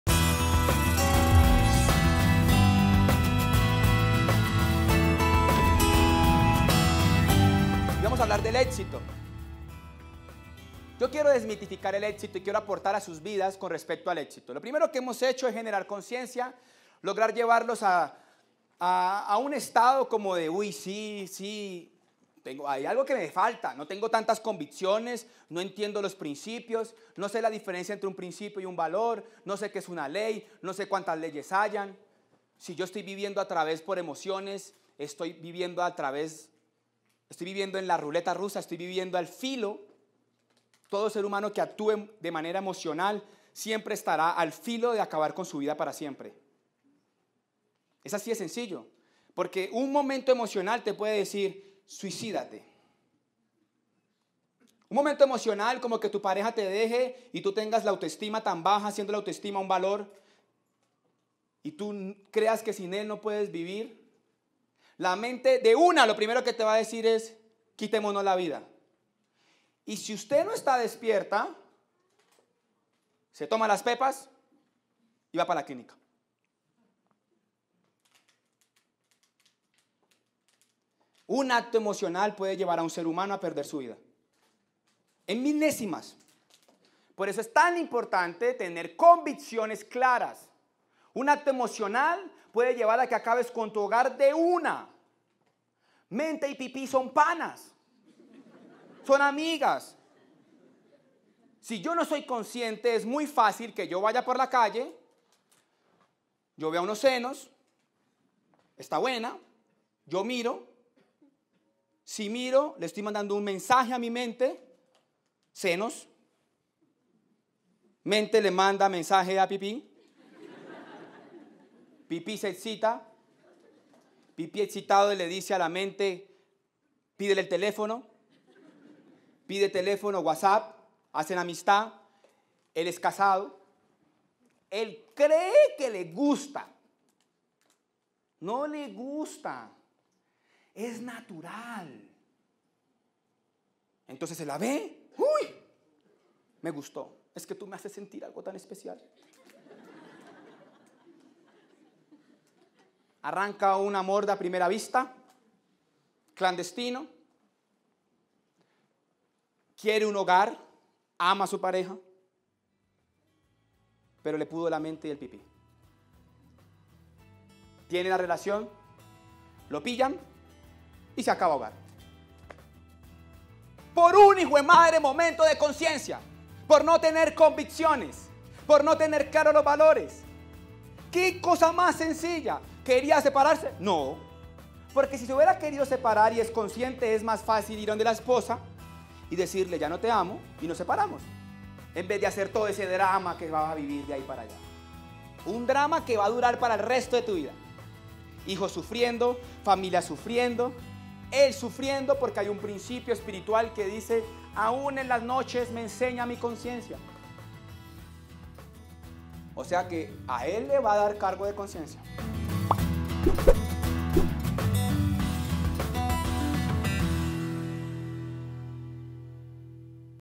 UN ACTO EMOCIONAL PUEDE SER FATAL - ANDRES LONDOñO
Un momento de emoción desconectado de la conciencia nos puede llevar a situaciones tan graves como un divorcio, la pérdida de un ser querido, la misma muerte, en fin. Por eso es importante actuar siempre desde la conciencia haciendo lo correcto.